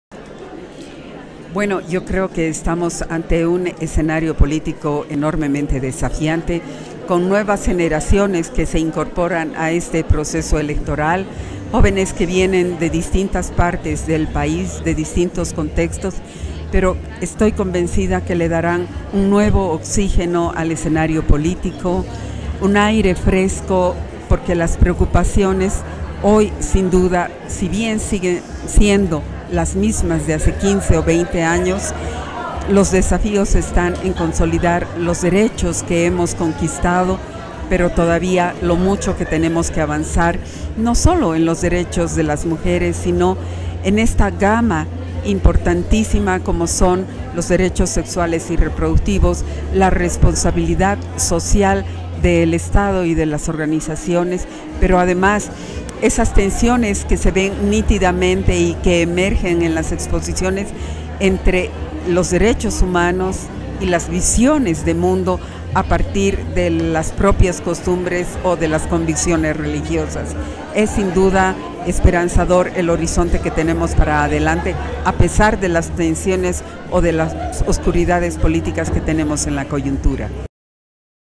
Primer Foro Nacional de Candidatxs: Protagonistas
Patricia Flores brinda sus impresiones, tras el primer foro que reunió candidaturas rumbo a las elecciones 2019.